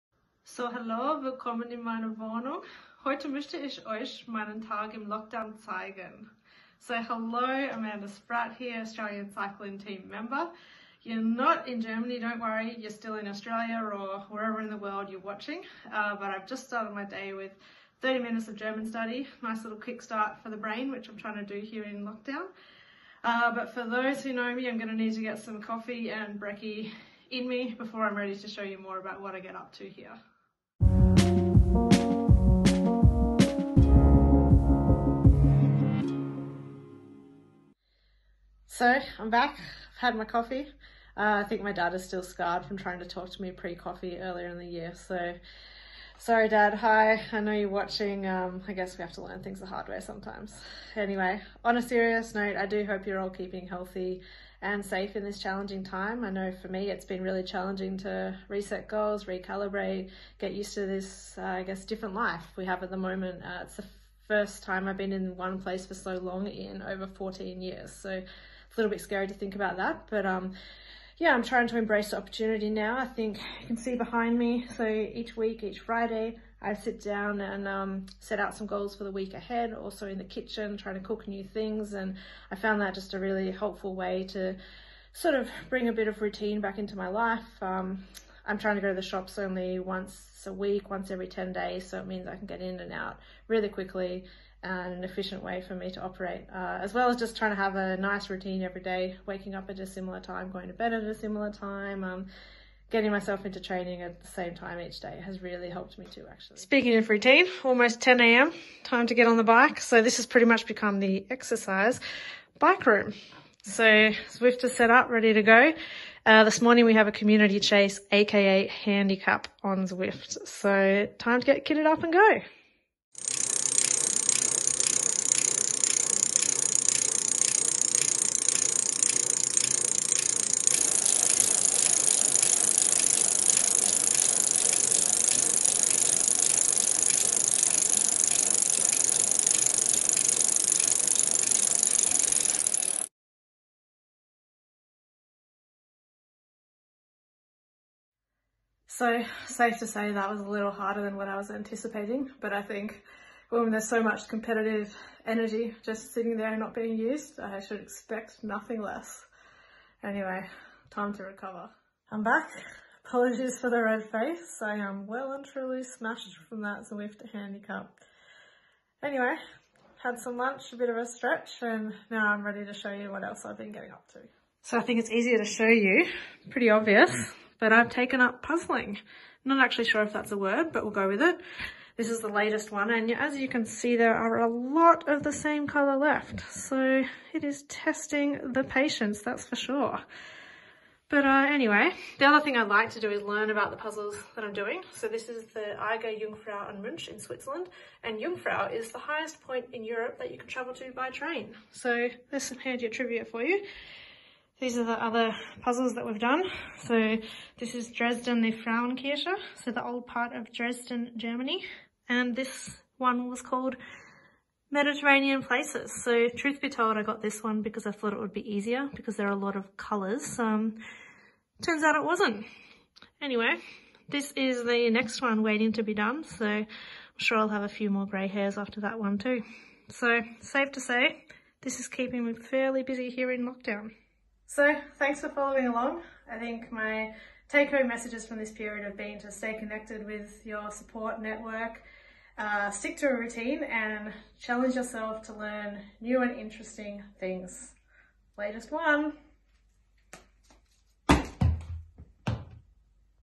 Amanda Spratt 30 from 30
The AusCyclingTeam’s ‘30 from 30 Series’ will feature current and former Australian Cycling Team athletes, coaches and staff across April who will show us the different, creative and unique ways they are keeping healthy and active both physically and mentally, and staying connected while in isolation or lockdown across Australia and the world.